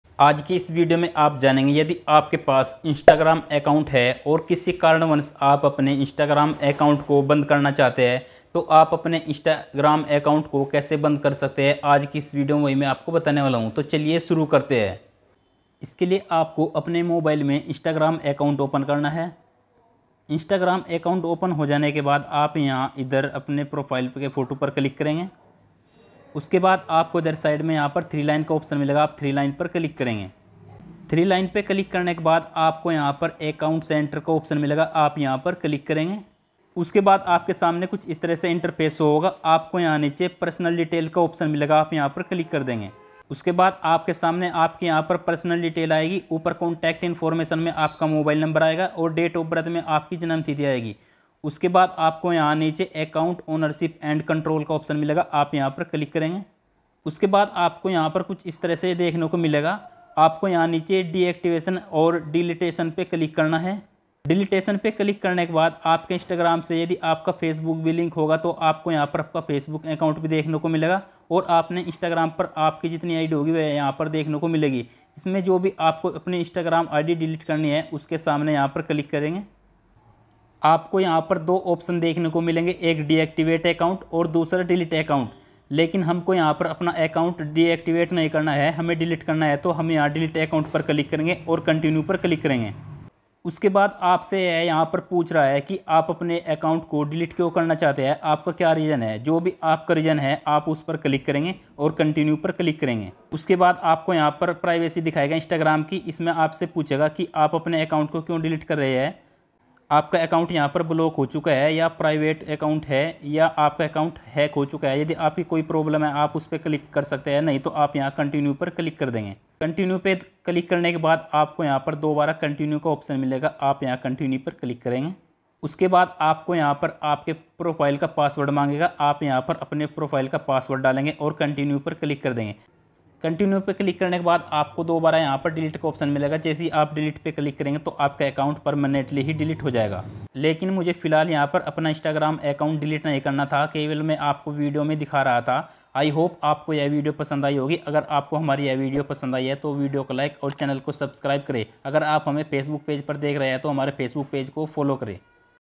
Instagram Account Delete Kaise kare | Permanently How To Delete Instagram Account | insta id delete
About Video - Hello Dosto Aaj ki es video me mene aapko bataya hai ki aap apne instagram id delete kaise kar sakte hai.